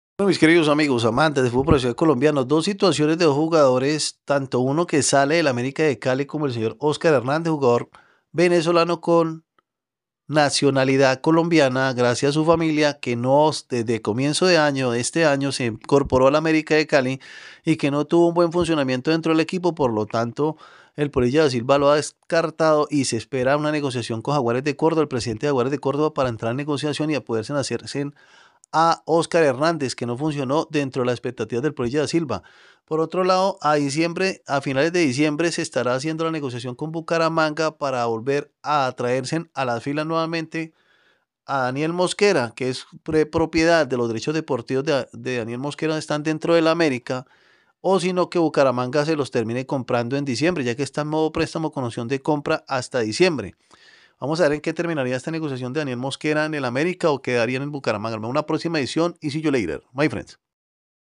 noticias: Dos jugadores son novedad en América de Cali
america podría pedir un jugador que es figura o venderlo al Bucaramanga y un jugador que sale de la institución